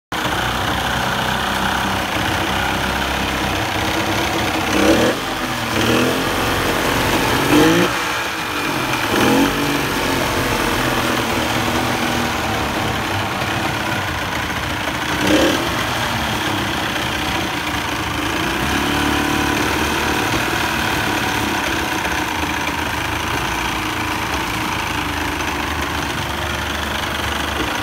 1998 Sweetwater Challenger Pontoon | For Sale | Online Auction | Repocast.com
1998 Sweetwater Challenger Pontoon | For Sale | Online Auction | Repocast.com
Selling via an Online Auction at Repocast.com

1998 Sweetwater Challenger 180ES Pontoon Boat. No Trailer, no battery, Key in Office, seats have some sunfade and discoloration in some spots, some dings and scratches along exterior, has individual seat covers for front benches and rear bench, Runs and Drives but starts hard and may need some repair, Titled Watercraft